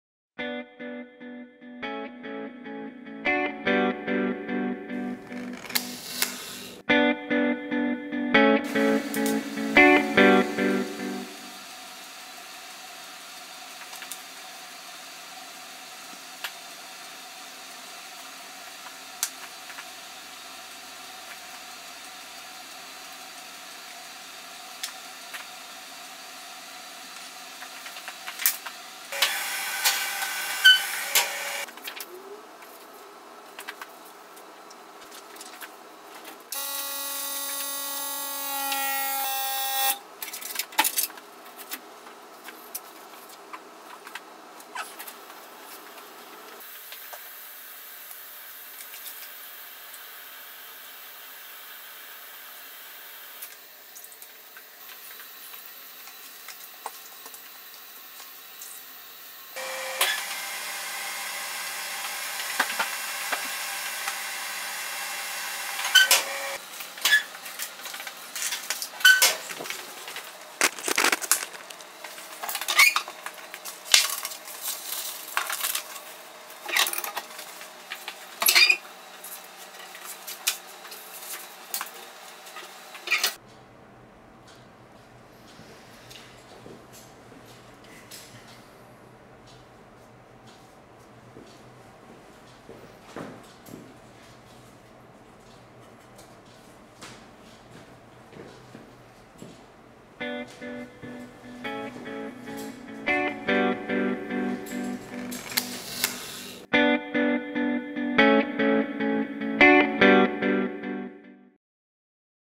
Vandyke brown contact print in time laps
This is a demonstration of  contact printing vandyke brown filmed in time lapse. This is not an instructional, see my other videos for clearer instruction.